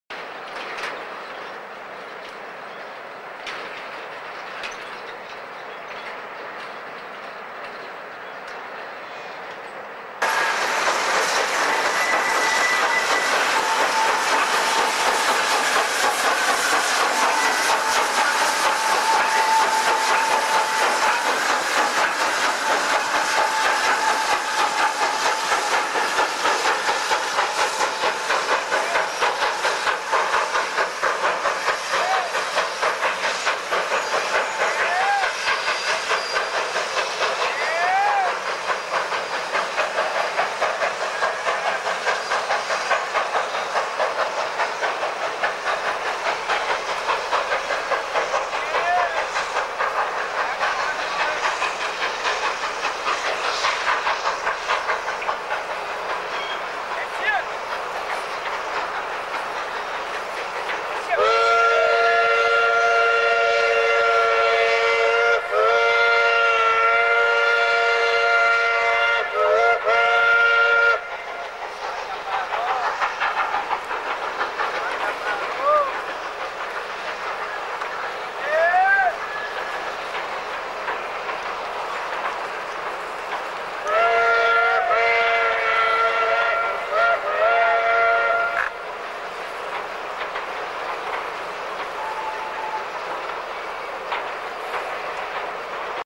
Amazingly well trained team of 10 Oxen in Cuba 1998
A team of 10 oxen handle a very heavy cart of sugar cane down a steep slope and across a railway crossing. The oxen need minimal instructions from their driver. The final pair of oxen act as brakes preventing the cart from running away down the slope. Filmed in April 1998.